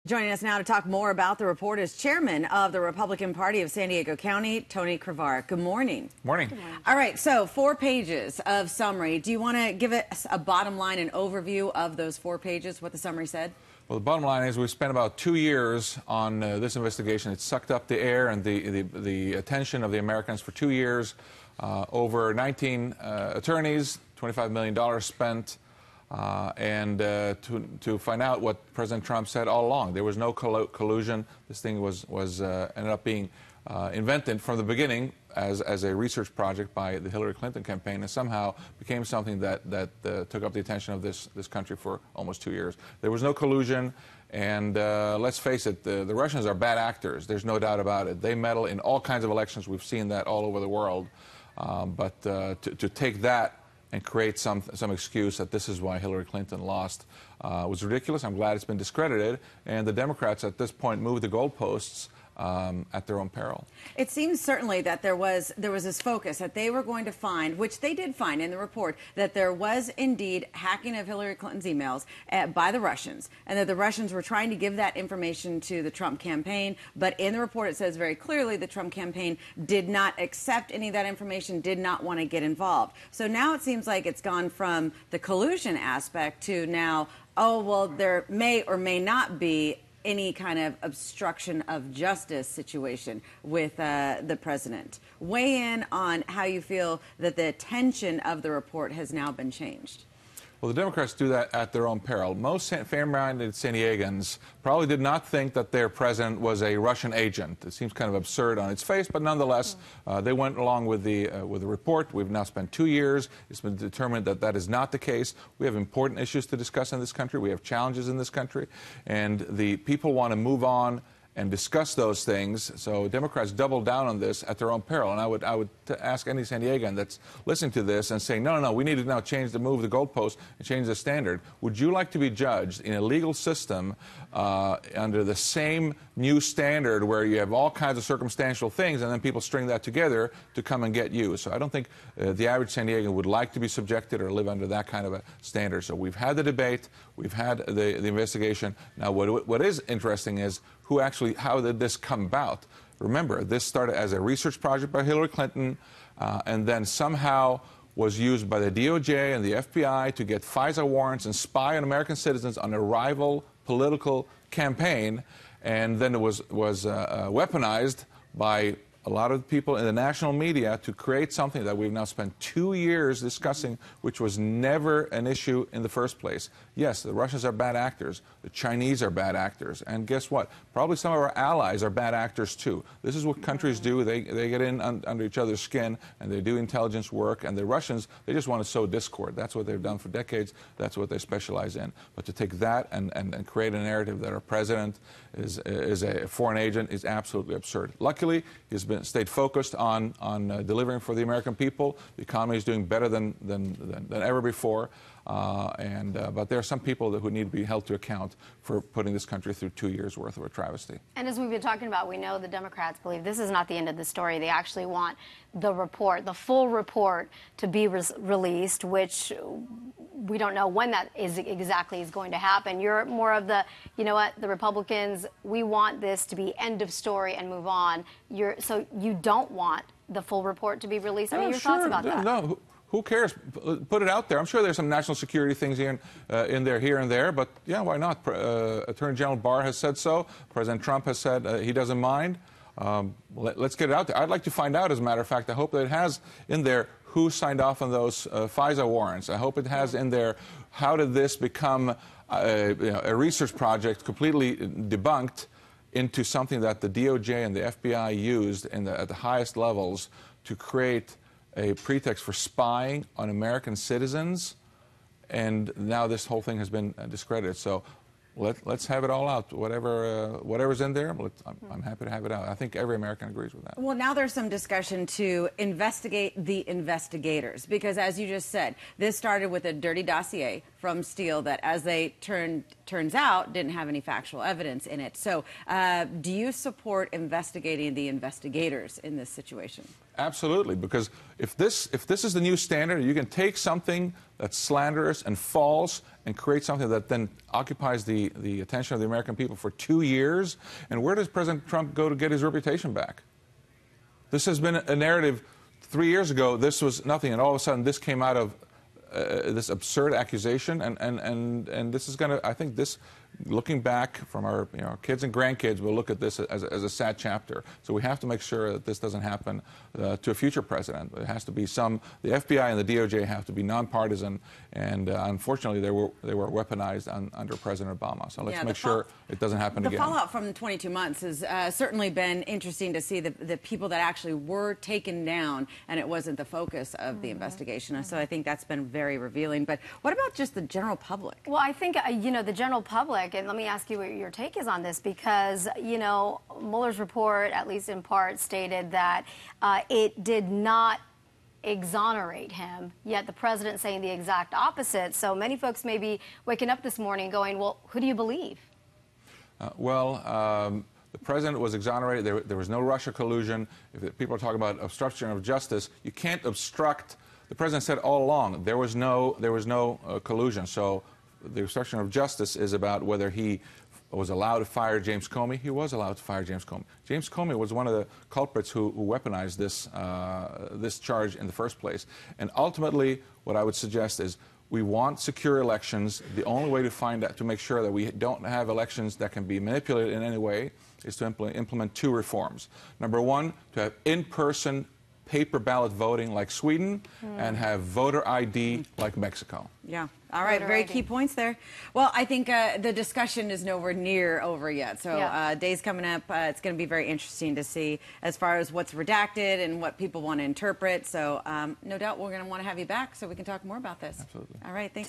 Chairman Tony Krvaric Reacts To The Mueller Report
Republican Party of San Diego County Chairman Tony Krvaric joined KUSI morning news to discuss the results of the long-awaited Mueller Report.